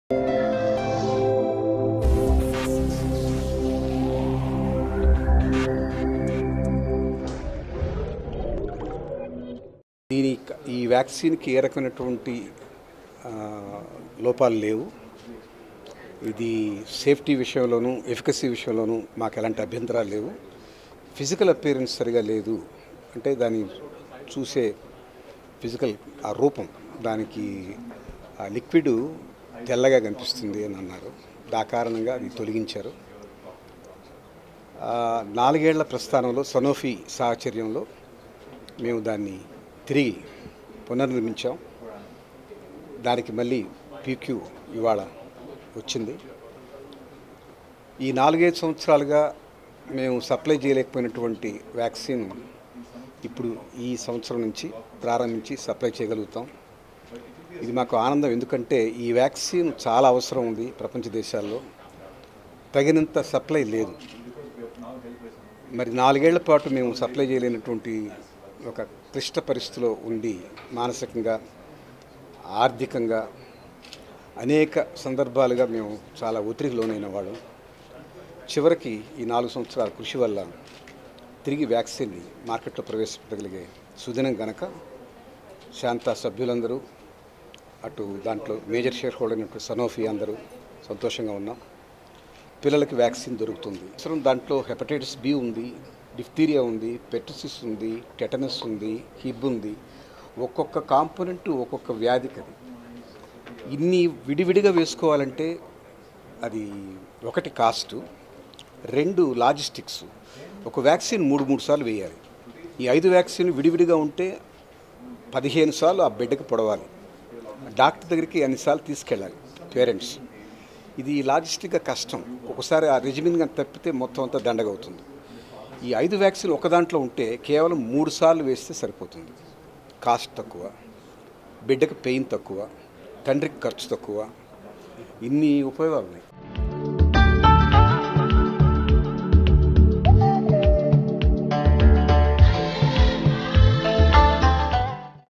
Dr K I Varaprasad About Benifits of Shan5
Dr. K.I. Varaprasad About benifits of Shan5 Vaccine